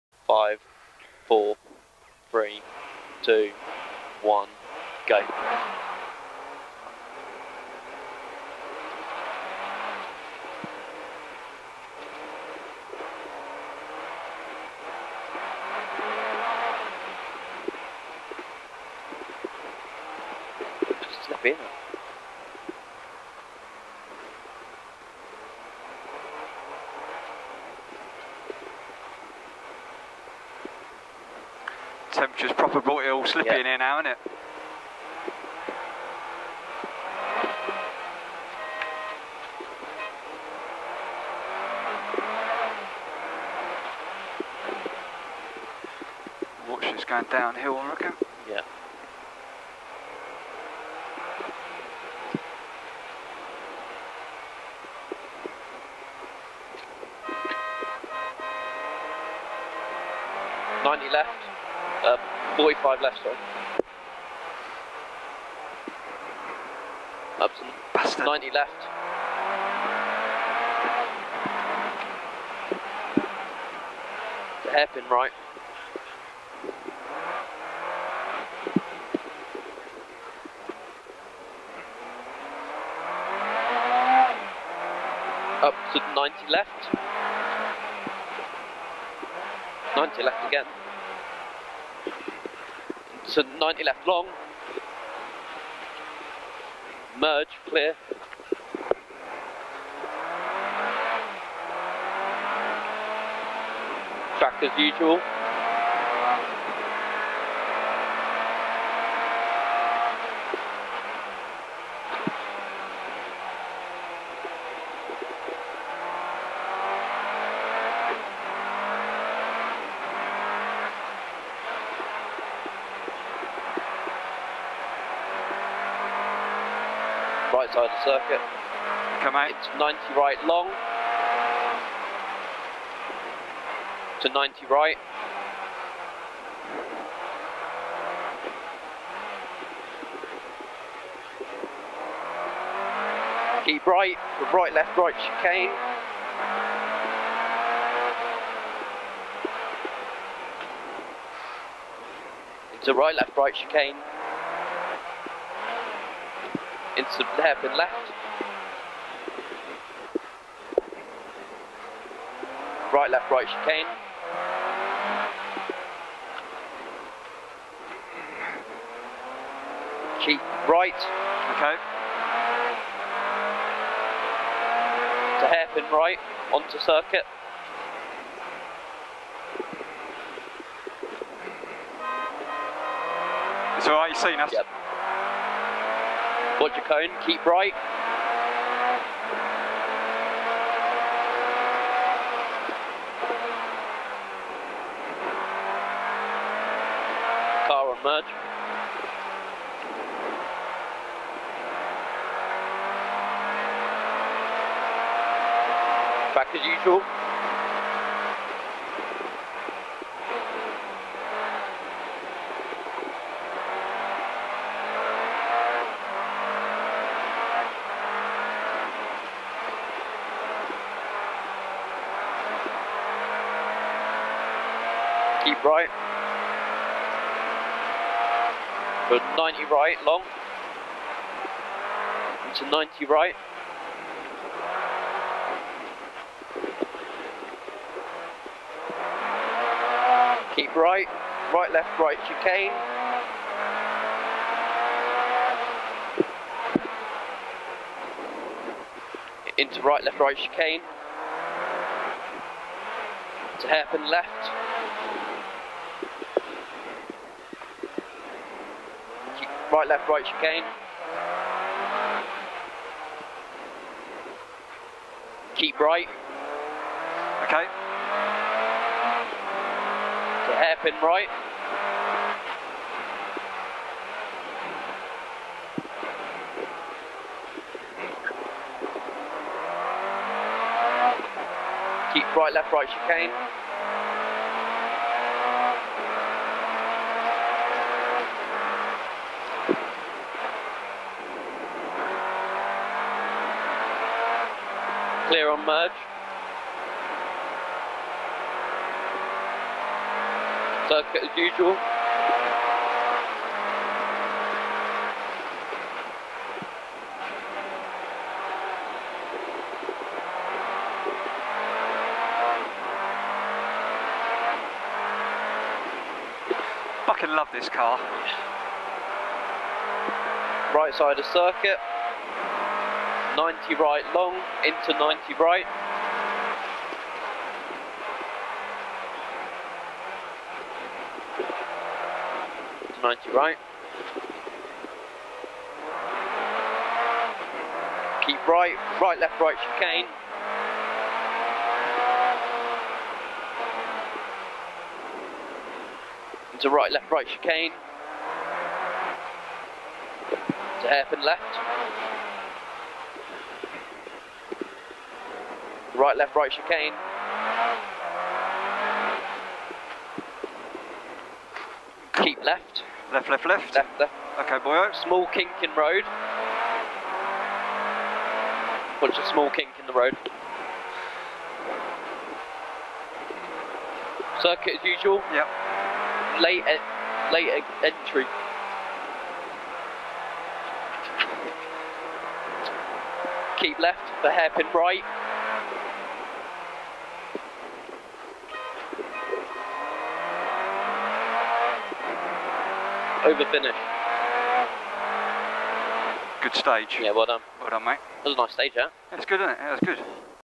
Ashley Davies & Fredy Hewitt Peugeot 205 GTI Mi16 Group A SS7 Brands Hatch Winter Stages
Really enjoyed this stage nice and flowing and Fredy really getting the hang of navigating giving good instruction.